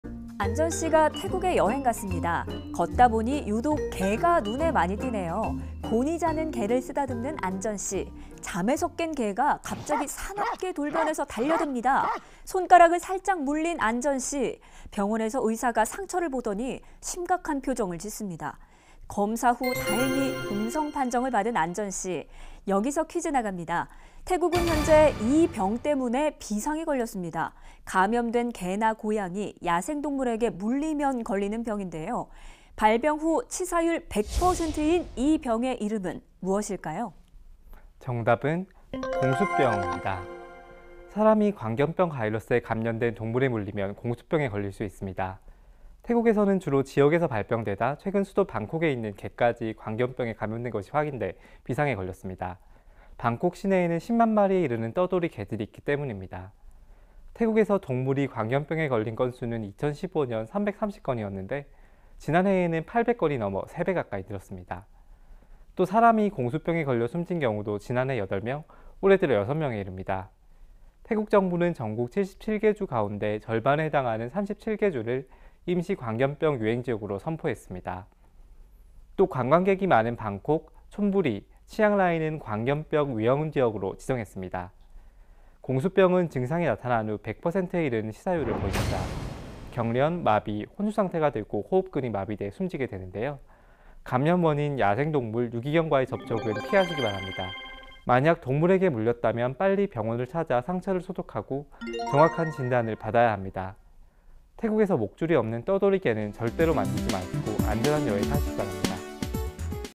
태국에서 개 건드리지 마세요…공수병·광견병 걸리면 치사율 100%! / YTN KOREAN
[앵커]
안전 씨가 태국에 여행 갔습니다.

걷다 보니 유독 개가 눈에 많이 띄네요.

곤히 자는 개를 쓰다듬는 안전 씨.

잠에서 깬 개가 갑자기 사납게 돌변해 달려듭니다.

손가락을 살짝 물린 안전 씨.

병원에서 의사가 상처를 보더니 심각한 표정을 짓습니다.

검사 후 다행히 음성판정을 받은 안전 씨.

여기서 퀴즈 나갑니다.

태국은 현재 이 병 때문에 비상이 걸렸습니다.

감염된 개나 고양이, 야생동물에게 '물리면' 걸리는 병인데요.

발병 후 '치사율 100%'인 이 병의 이름은 무엇일까요?

[전성민 사무관 ]
정답은 공수병입니다.

사람이 광견병 바이러스에 감염된 동물에 물리면 공수병에 걸릴 수 있습니다.

태국에서는 주로 지역에서 발병되다가 최근 수도 방콕에 있는 개까지 광견병에 감염된 것이 확인돼 비상이 걸렸습니다.

방콕 시내에는 10만 마리에 이르는 떠돌이 개들이 있기 때문입니다.

태국에서 동물이 광견병에 걸린 건수는 2015년 330건이었는데, 지난해에는 8백 건이 넘어 세 배 가까이 늘어났습니다.

또 사람이 공수병에 걸려 숨진 경우도 지난해 8명, 올해 들어 6명에 이릅니다.

태국 정부는 전국 77개 주 가운데 절반에 해당하는 37개 주를 임시 광견병 유행 지역으로 선포했습니다.

또 관광객이 많은 방콕, 촌부리, 치앙라이는 광견병 위험지역으로 지정했습니다.

공수병은 증상이 나타난 후 100%에 이르는 치사율을 보입니다.

경련, 마비, 혼수상태가 되고 호흡근이 마비되어 숨지게 되는데요.

감염원인 야생동물, 유기견과의 접촉은 피하시기 바랍니다.

만약 동물에게 물렸다면 빨리 병원을 찾아 상처를 소독하고 정확한 진단을 받아야 합니다.

태국에서 목줄이 없는 떠돌이 개는 절대로 만지지 마시고 안전 여행하시길 바랍니다.